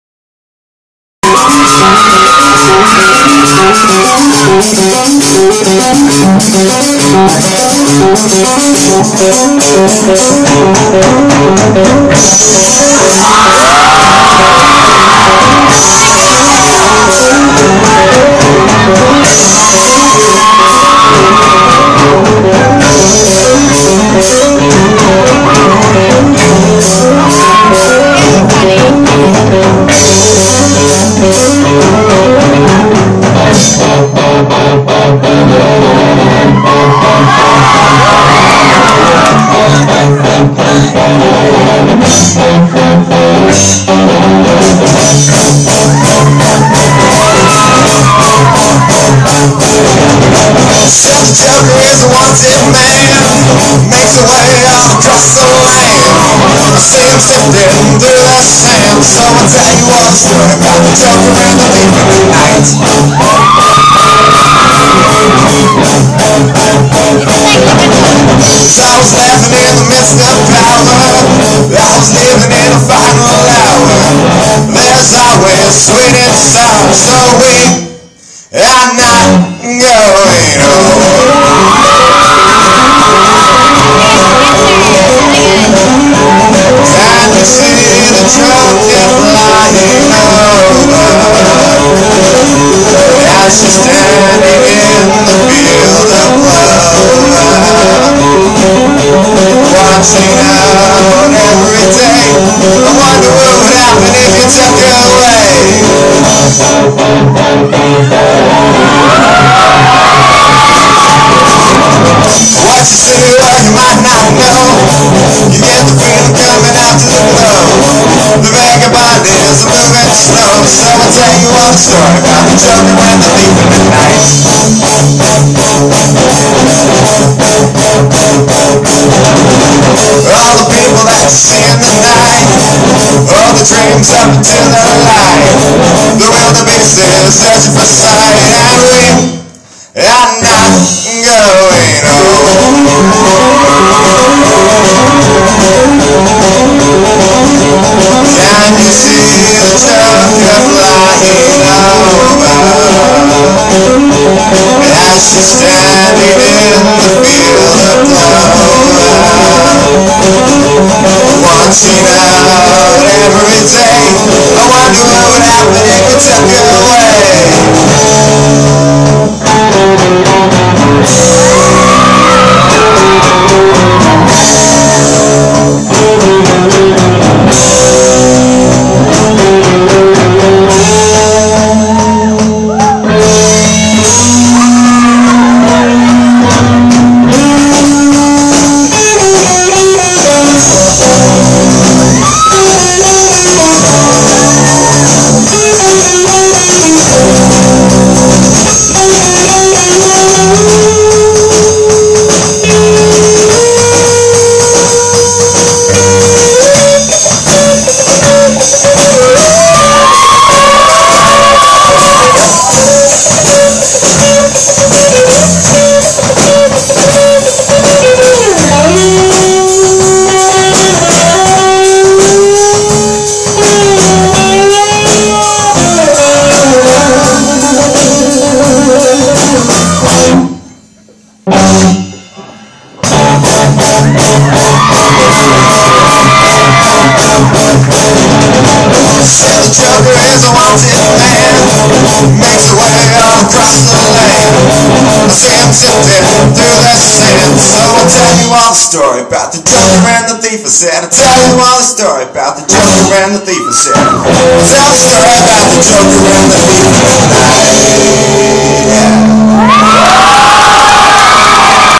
Brees Vlog #20 Father Lacome High School Coffee Shop 2011
Kris and his band